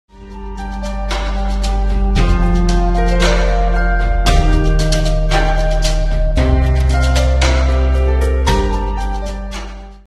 VW Caddy Camper DJI Fly Around
Just a quick fly around of the Caddy Camper using a DJI Mavic Air !